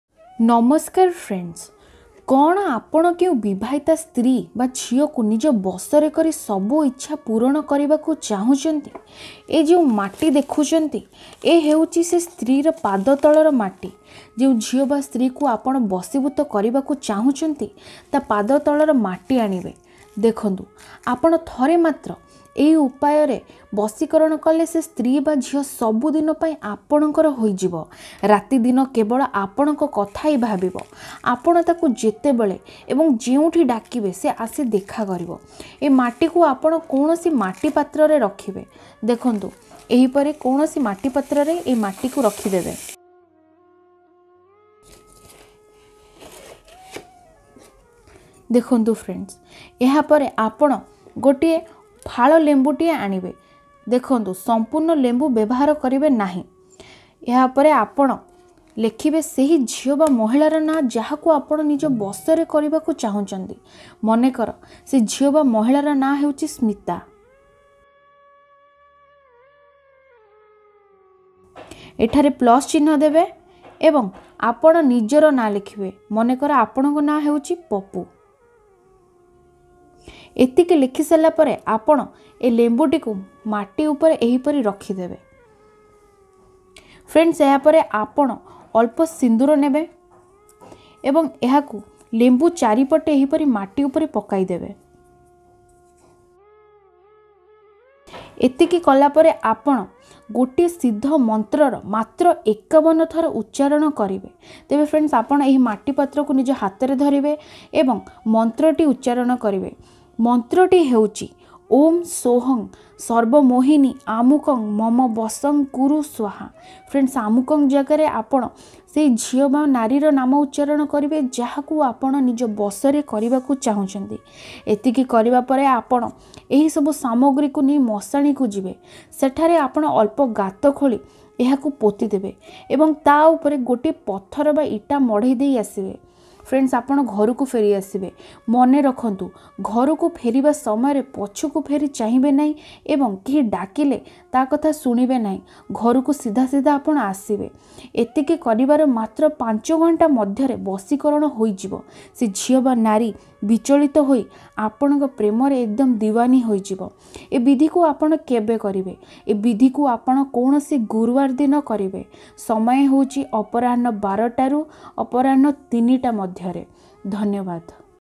ଲେମ୍ବୁରେ ଝିଅ ପଟେଇବା ବଶୀକରଣ ଶିଖନ୍ତୁ | odia vashikaran
Namaskar friends ajira ehi video tire mu apanaku nimbu se vashikaran kipari hue se bisayare kahichi jahaku prayaga kari apana pasand karuthiba kounasi jhiaku patei paribe paribe